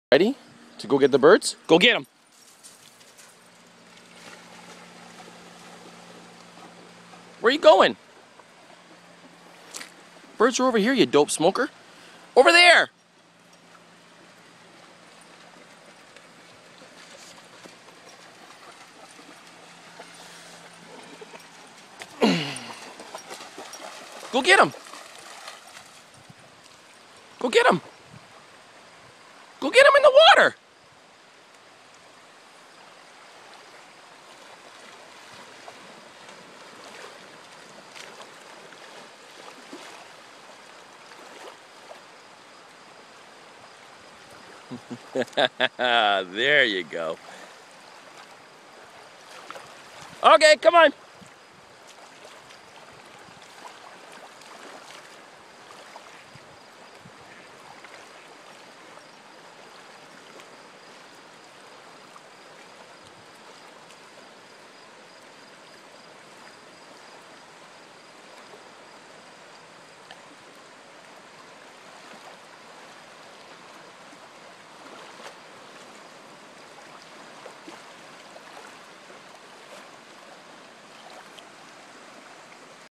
Little Tiny Niko chases ducks
My puppy likes to chase the birds!